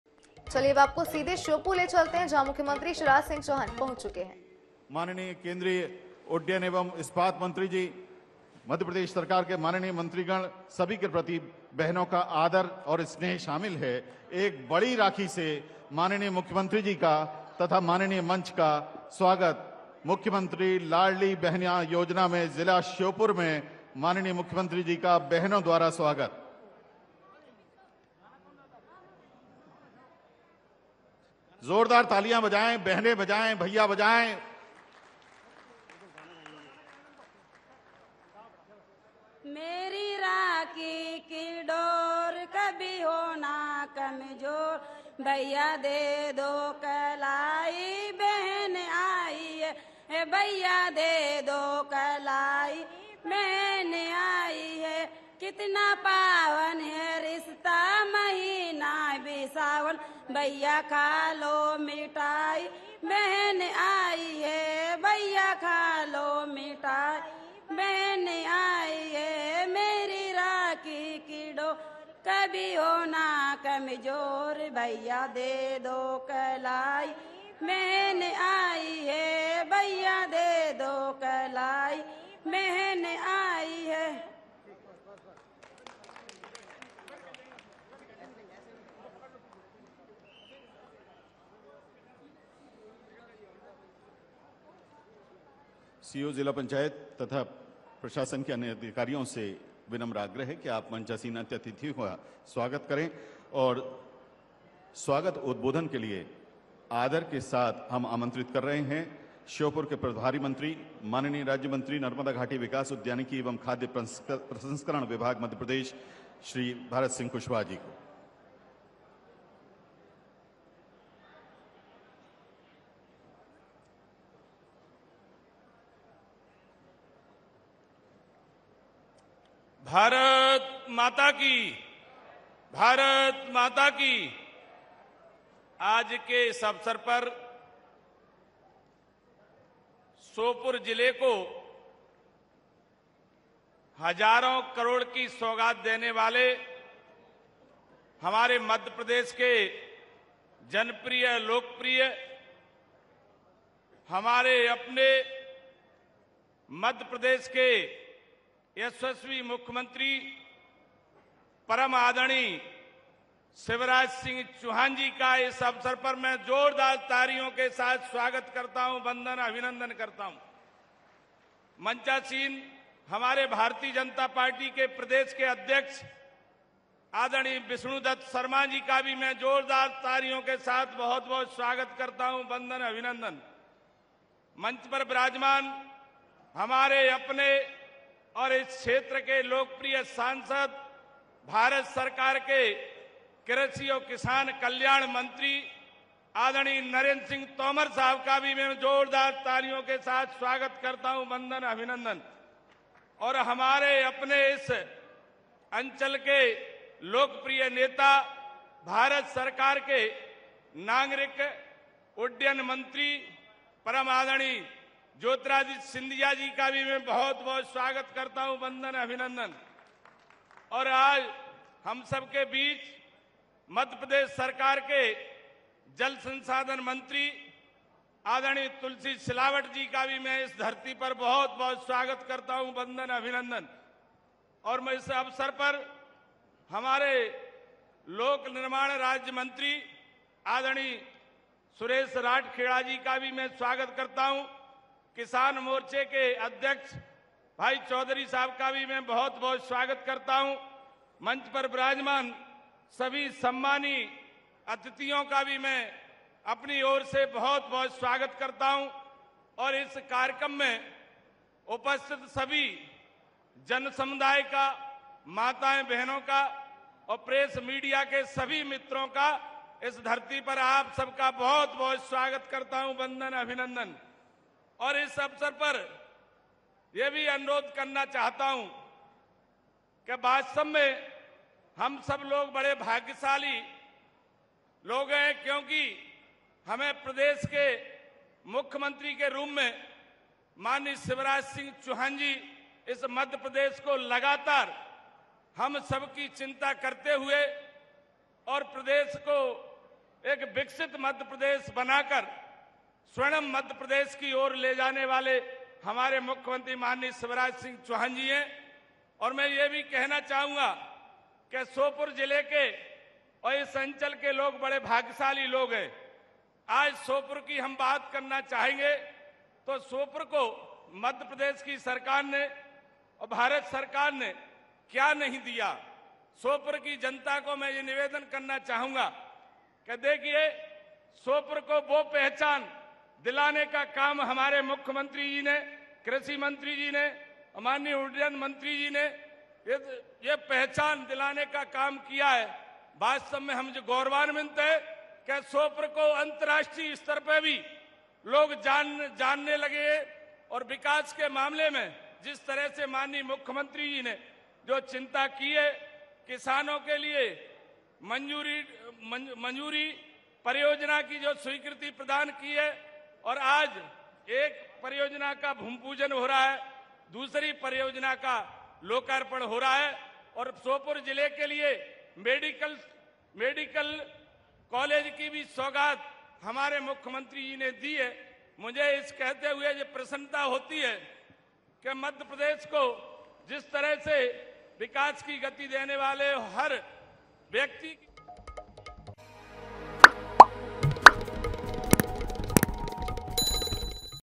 Sheopur LIVE : CM Shivraj Singh Chouhan ने खोला सौगातों का पिटारा, जानें किसके लिए क्या लाए मामा ?
INH, Sheopur LIVE : CM Shivraj Singh Chouhan ने खोला सौगातों का पिटारा, जानें किसके लिए क्या लाए मामा ?